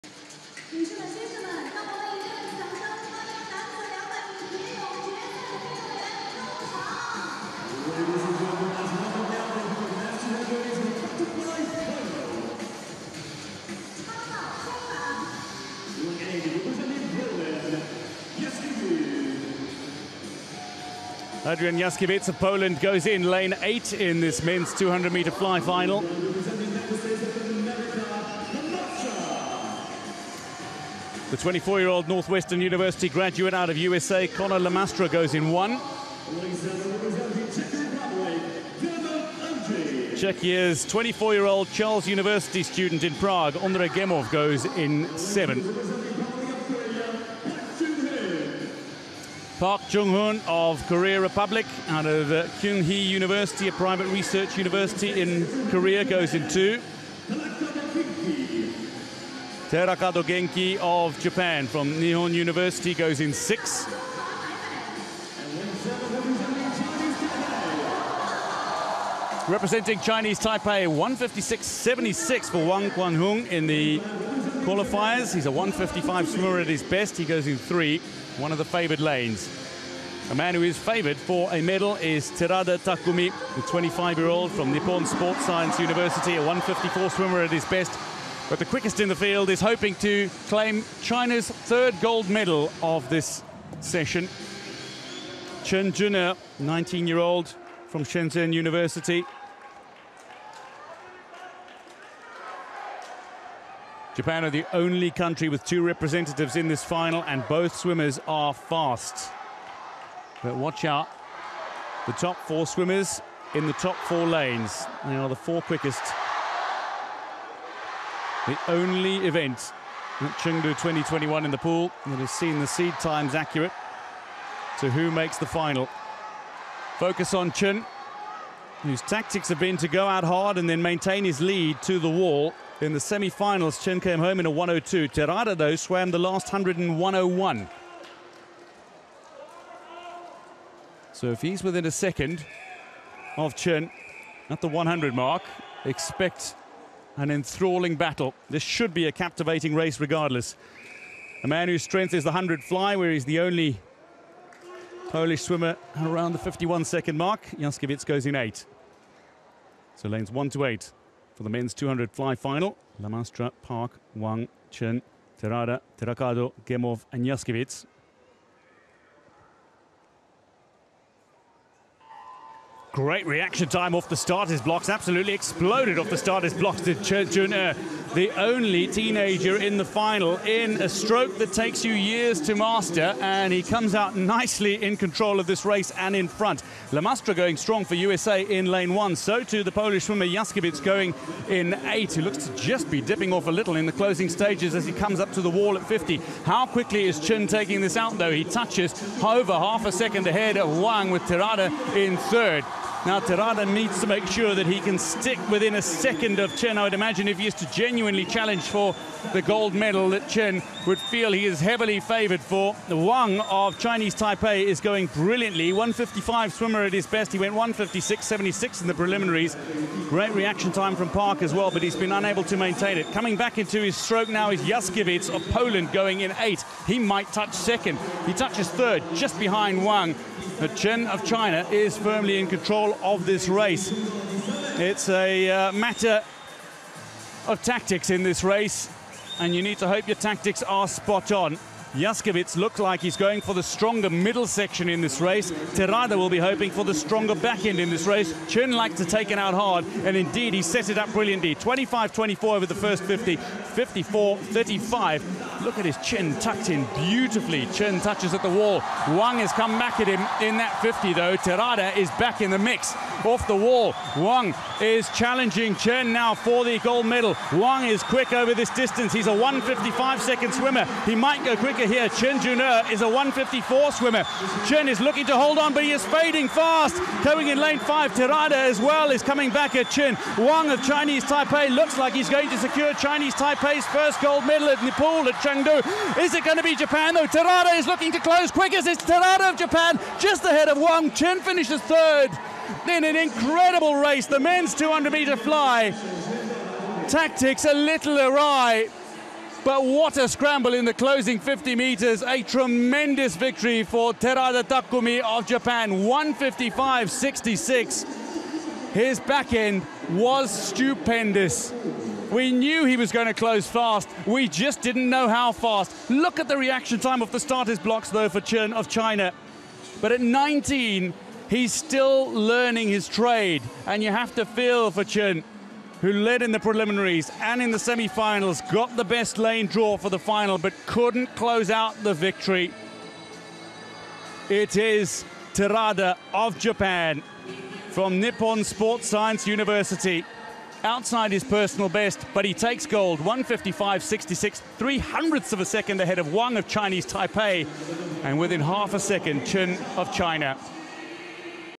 LSUH 2023 - 200 M - finále + český rekord - Ondřej Gemov
Ondřej Gemov z Univerzity Karlovy přidal na Letních světových univerzitních hrách další finále, pro české barvy již čtvrté. Ve závodě na 200 metrů motýlek se mu podařilo vylepšit rekord ČR výkonem 1:56,91 a v celkovém pořadí vybojoval 5. místo.